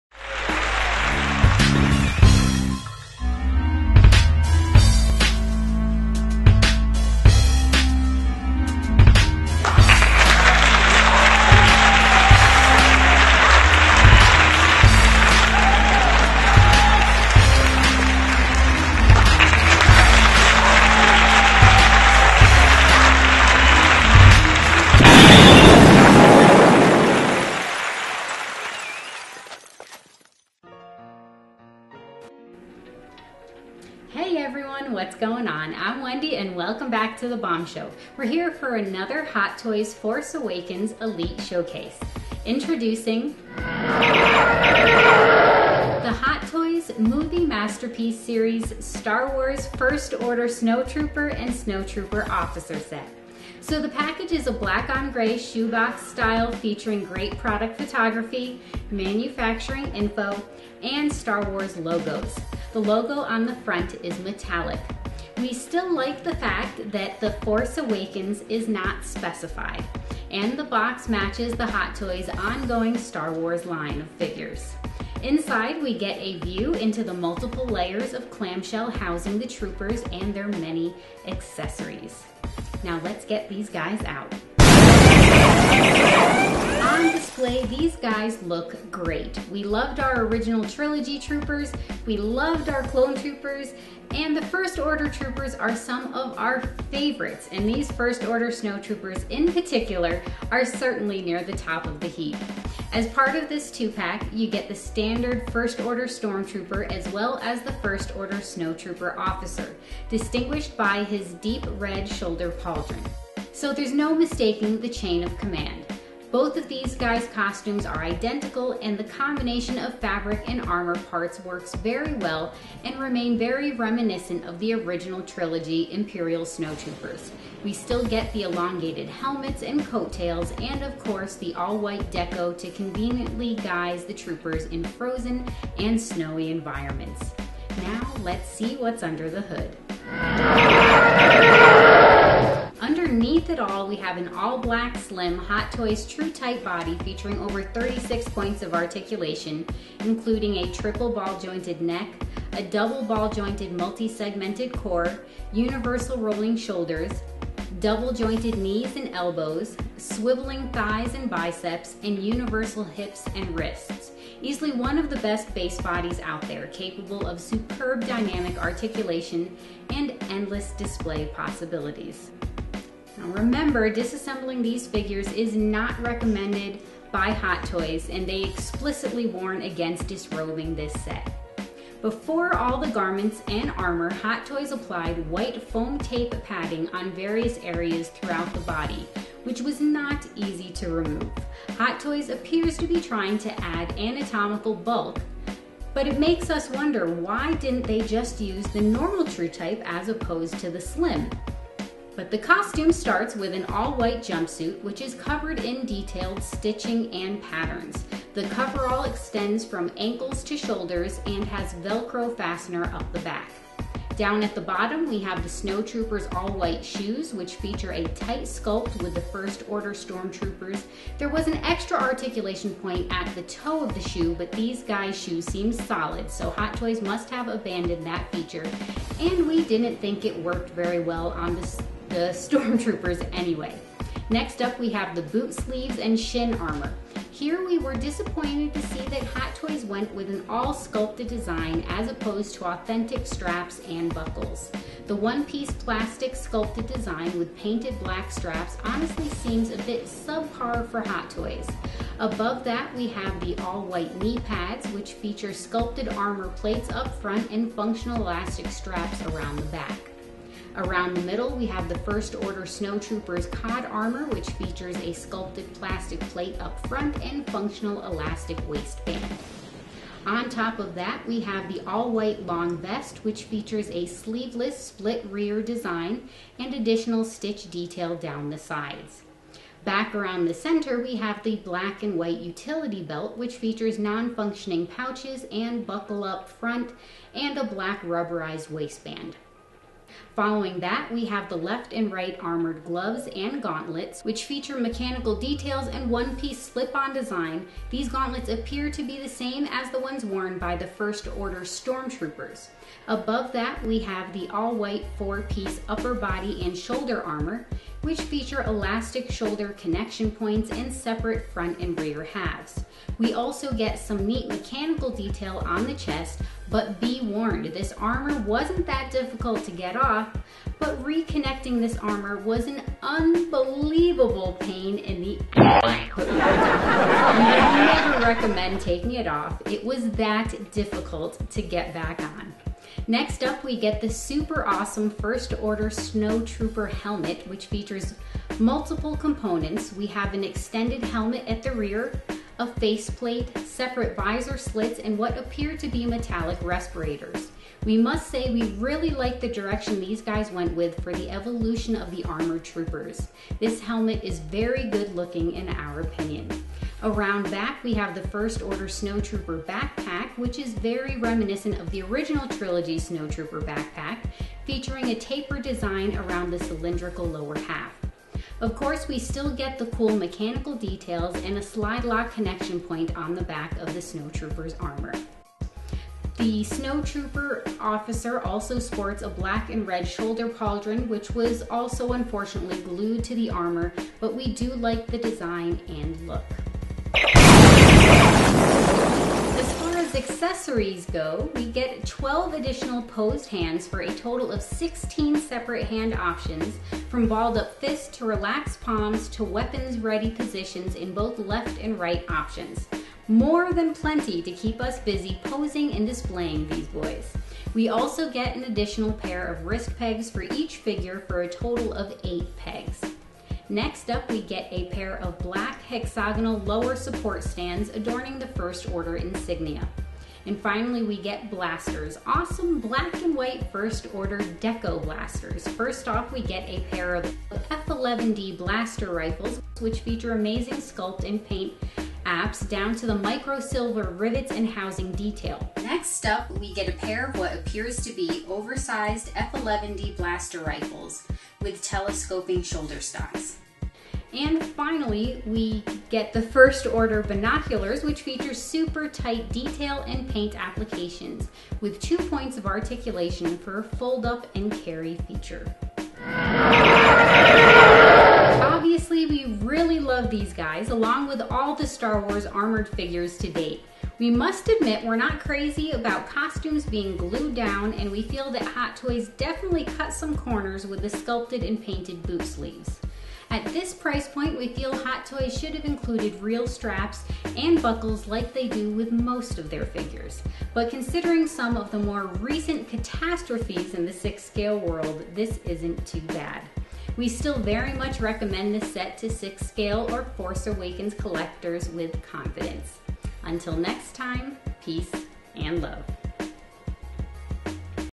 Hot Toys First Order Snow Troopers 1/6 Figure Set - Da Bomb Show Elite Showcase
In this Da Bomb Show Elite Showcase host Wendy covers the First Order Snow Trooper and Snow Trooper Officer Set by Hot Toys from the blockbuster film Star Wars: The Force Awakens.